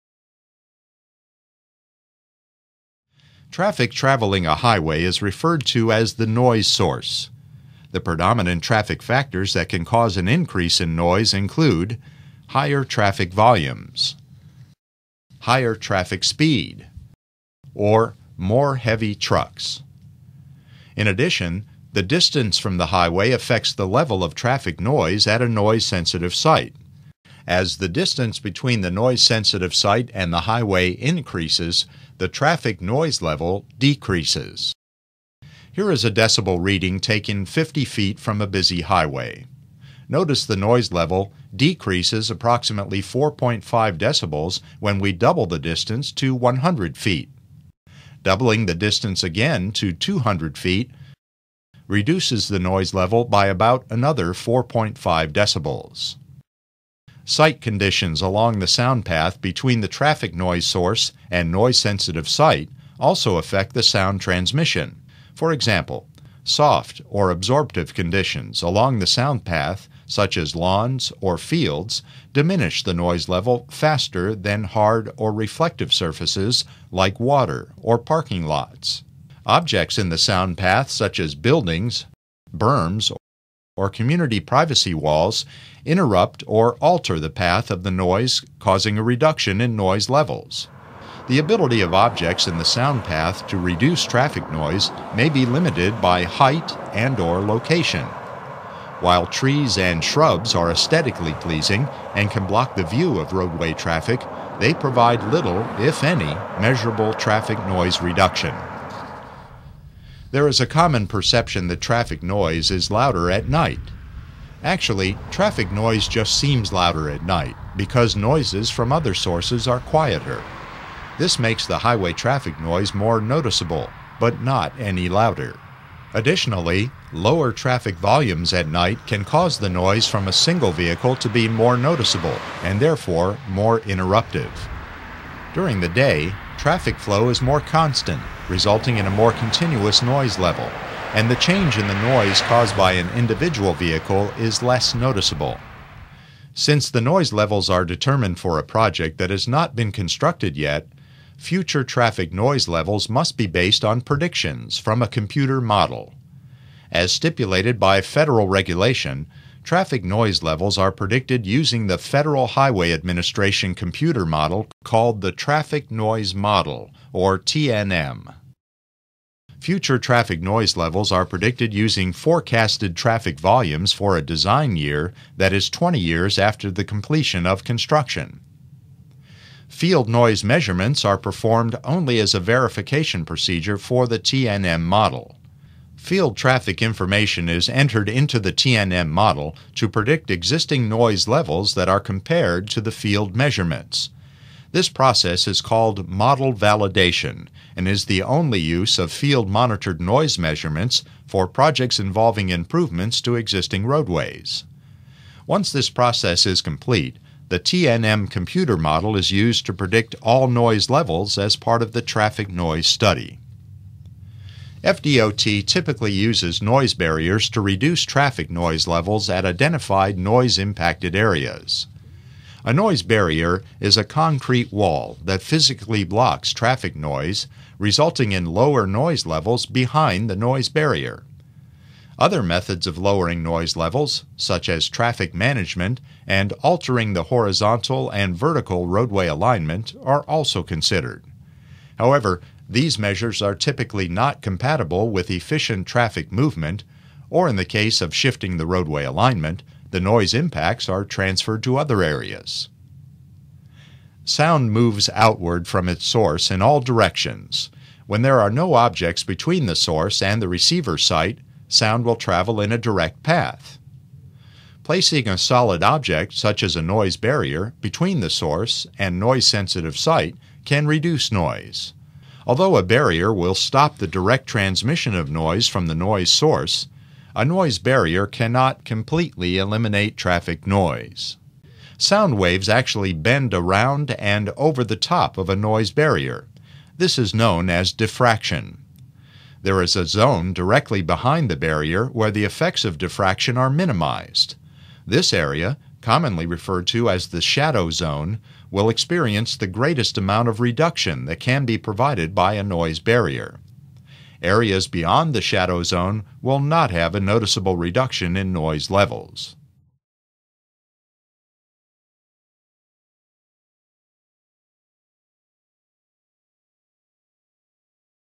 Sound Barrier Wall Part 2
Ever want to know how FDOT determines if a new construction area receives a sound barrier? This three part video will provide you with those answers and more.